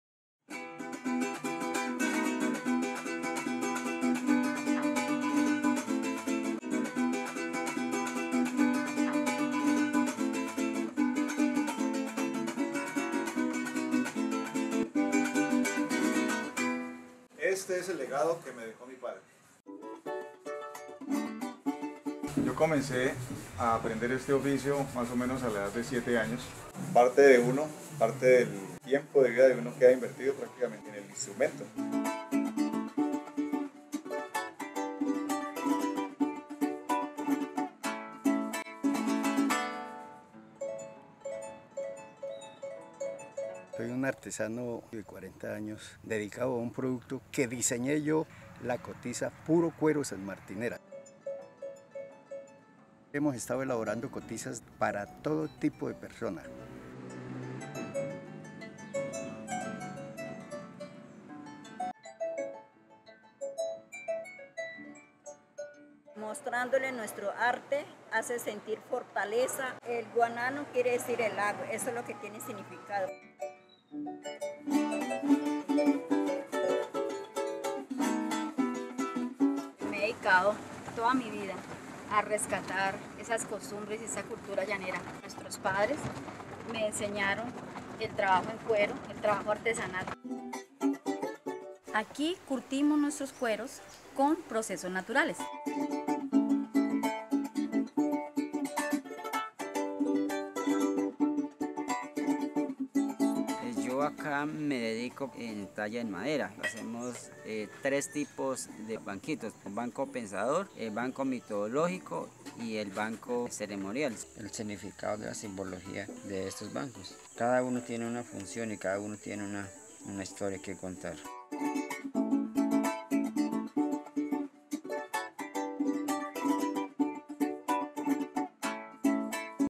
Colombia Artesanal: Ruta Meta
Pensar en Llano, en la imagen de la llanura. Pisar los Llanos orientales. A eso te invitamos al imaginarnos esta ruta por el territorio del horizonte infinito. Será la oportunidad para conocer a su gente, orgullosa de su territorio, de su legado campesino y atado a la tierra. Una tierra a la que se le canta y trova, que se cabalga y se le zapatea al son del joropo. Aquí verás cómo los oficios artesanales reflejan una idiosincrasia y una cultura arraigadas, en donde una poderosa cultura ganadera ha hecho del trabajo en cuero un sello de identidad, tan notable como la elaboración de cotizas con las que se baila y camina o como el chinchorro que le ofrece descanso a sus hijos.